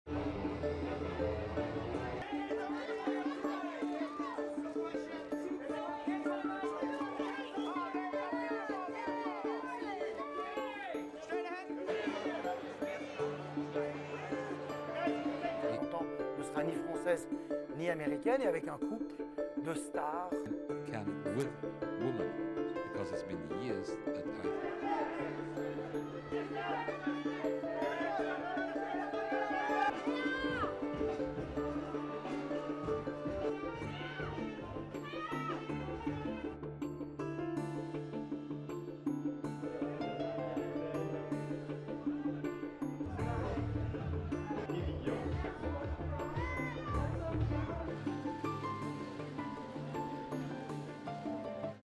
ARCHIVES Cannes 2018, un jury majoritairement féminin
C'est un jury majoritairement féminin qui choisira la prochaine Palme d'or, lors du premier Festival de Cannes depuis les révélations sur le harcèlement dans le 7e art et le séisme de l'affaire Weinstein. Elles seront 5 femmes, pour 4 hommes, dans ce jury présidé par l'actrice Cate Blanchett. IMAGES D'ARCHIVES de Kristen Stewart, Léa Seydoux, Chang Chen, Denis Villeneuve, Robert Guédiguian et Andreï Zviaguintsev, tous membres de ce jury - Durée: 00:52